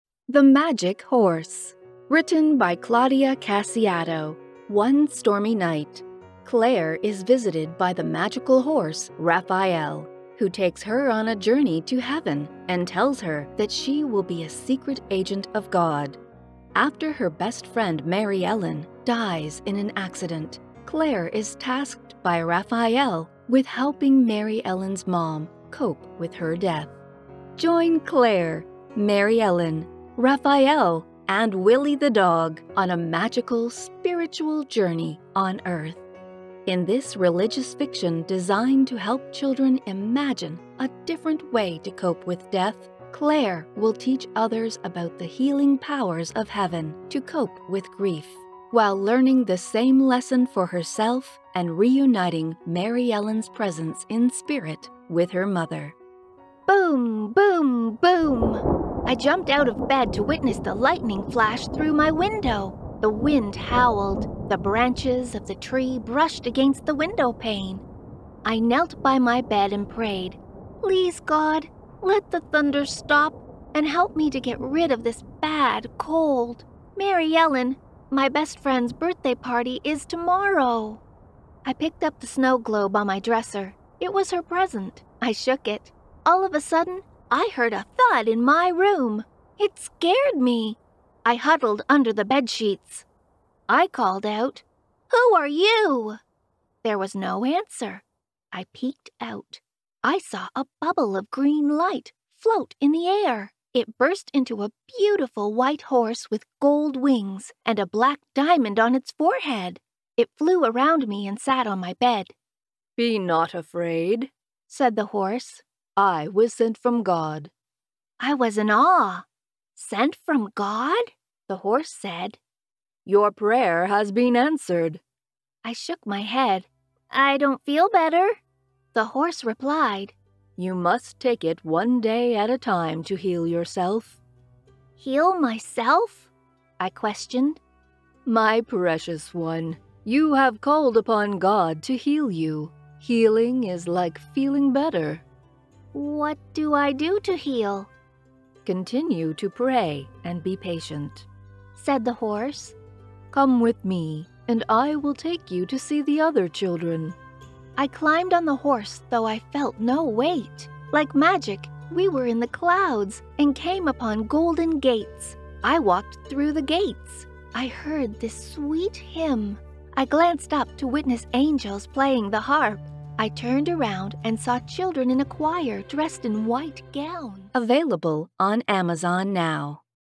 Children's Book Trailer
Our studio not only provides voiceovers but also publishing and video services as well.  We designed this sweet book trailer for Claudia Casciato to promote her new children's book to help children deal with a very sensitive topic death.  We are proud to be a part of this production that hopefully will help our children.

Usage Notice: All content in this video, including graphics and audio, is protected by copyright.